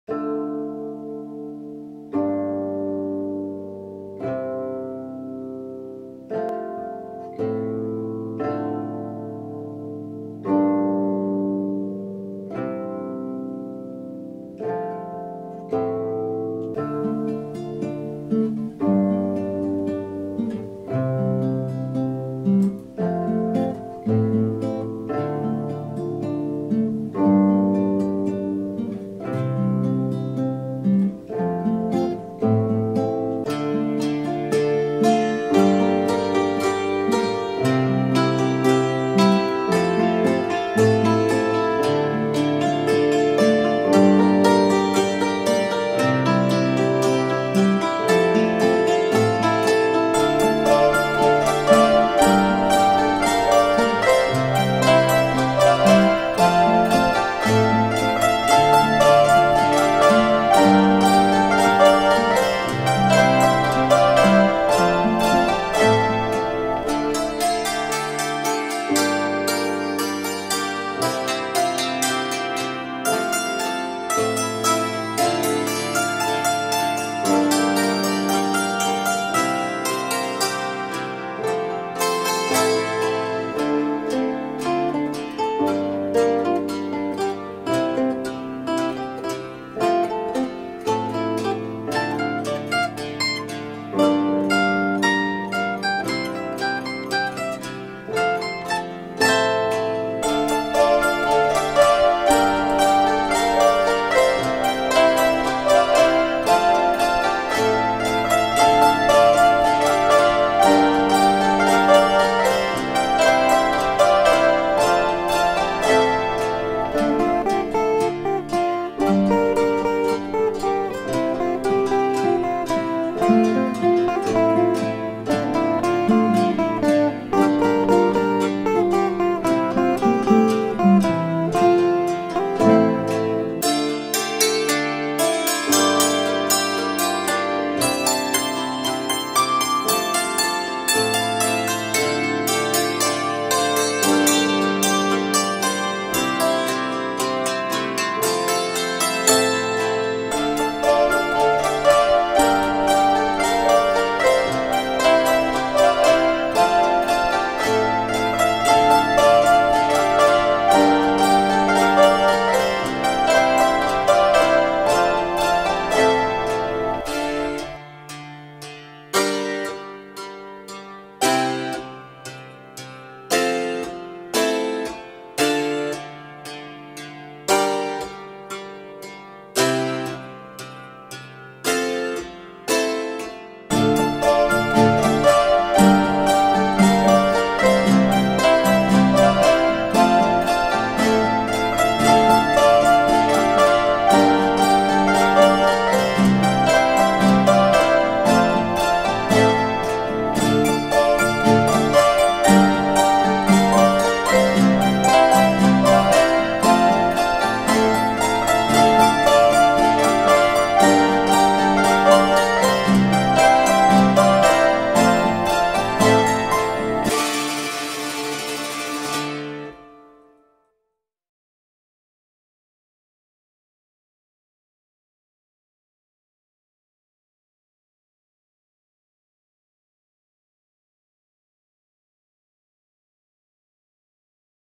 Saitenjugend 2016
Dieses Musikvideo ist im Oktober 2016 auf einem viertägigen Jugendseminar des Landes-Hackbrett-Bunds Baden-Württemberg e.V. entstanden. Die Jugendlichen haben die Komposition selbst eingespielt und durch eigene Solos erweitert. Außerdem wurde das Videomaterial von den Jugendlichen gedreht und geschnitten.